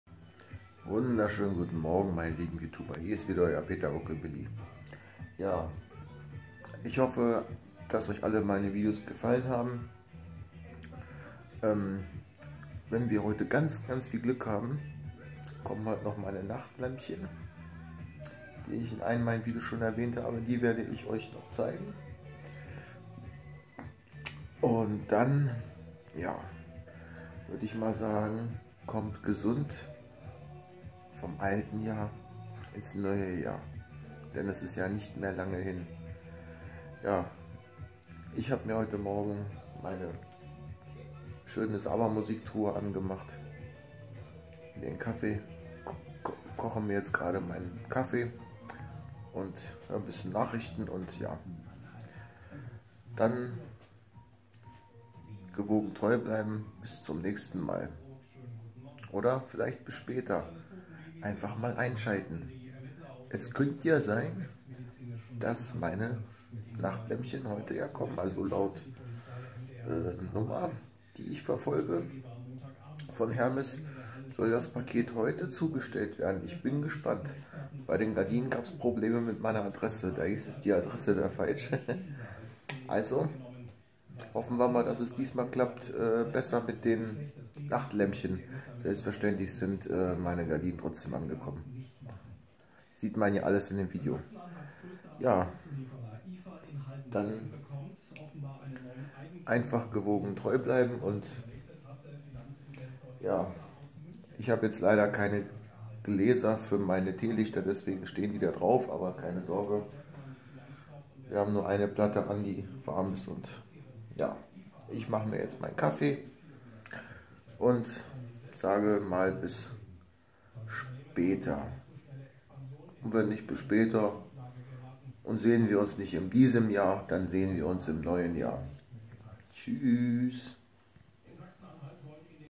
Guten Morgen noch mal kurzes Update von mein Museum.
50er und 60 er Jahre Hobbyheim Museum (Lebhaft) by Peter Rockabilly
Auf diesem Kanal findet ihr alles rund rum meiner Hobbys. Bliebt dran.

Musiktruhe Blaupunkt
Wasser kocher
Toaster
Cocktail Sessel
Designer Sofa,Sessel
Steh Lampe
Deko Artikel
LP. Schallplatten
Küchen Stühle Tisch
Nierentische
usw..
(Aber ich bin noch nicht ganz fertig)